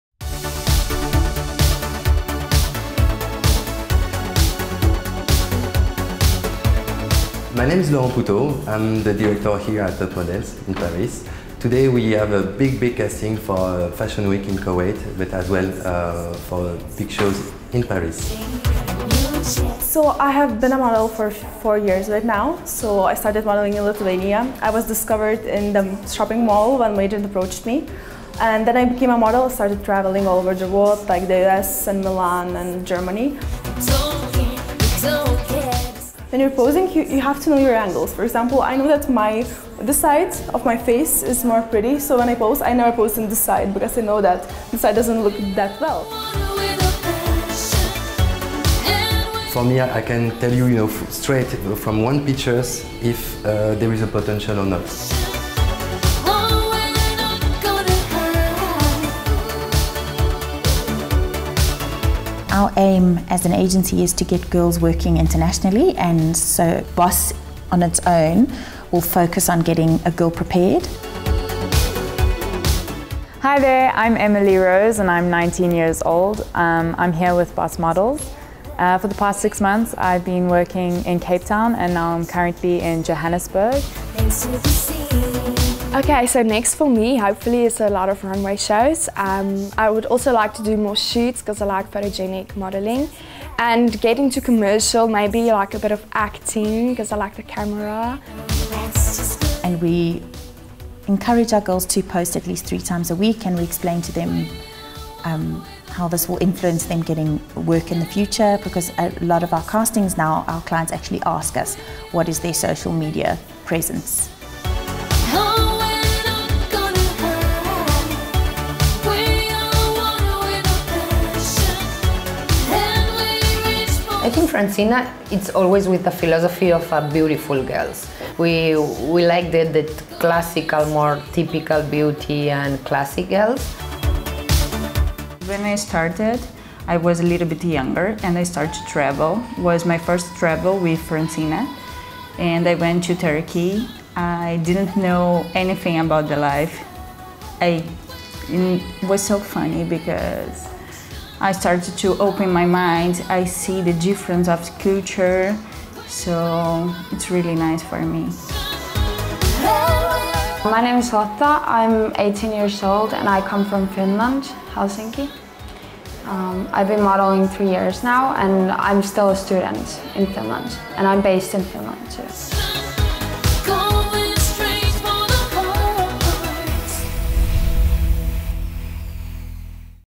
AGENCIES | Leading Model Agencies from Paris, Johannesburg and Barcelona
Fashion capitals pride themselves for being influencers in the industry thus expectations from their players are high. These agencies are up for the challenge and have proven to be an indispensable source of talents. Let’s see what’s a day in the busy life inside UP Models in Paris, Boss Models in Johannesburg, and Francina Models in Barcelona.